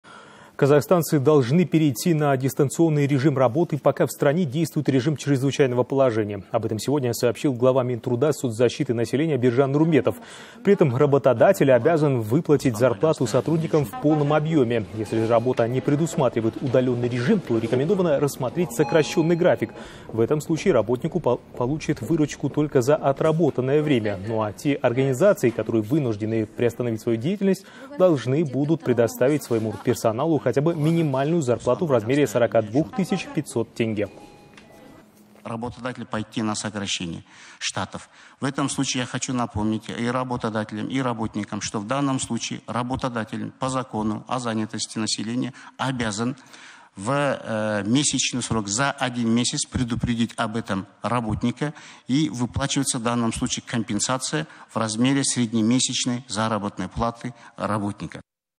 Казахстанцы должны перейти на дистанционную работу - Нурымбетов (16.03.20)
Казахстанцы должны перейти на дистанционный режим работы, пока в стране действует режим чрезвычайного положения. Об этом сегодня сообщил глава Минтруда и соцзащиты населения Биржан Нурымбетов, передает Almaty.tv.

При этом работодатель обязан выплатить зарплату сотрудникам в полном объеме. Если же работа не предусматривает удаленный режим, то рекомендовано рассмотреть сокращенный график. В этом случае работник получит выручку только за отработанное время. А те организации, которые вынуждены приостановить свою деятельность, должны будут предоставить своему персоналу хотя бы минимальную зарплату в размере 42500 тенге.

«Работодатель может пойти на сокращение штата. При этом работодатель обязан заранее, за один месяц предупредить об этом работника. В данном случае выплачивается компенсация в размере среднемесячной зарплаты работника», - сообщил министр труда и социальной защиты населения РК Биржан Нурымбетов.